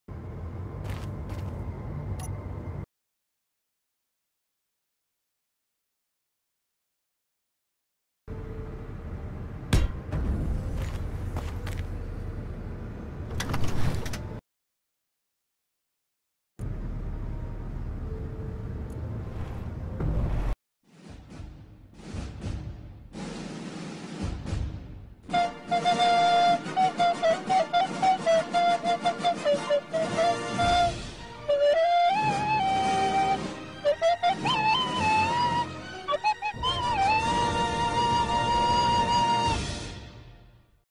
How to Build a Rover in 40 Seconds
A quick run through on how to build a rover from a battery lying on the ground. Helpful for using the auxiliary batteries in the Immersive Alternate Start: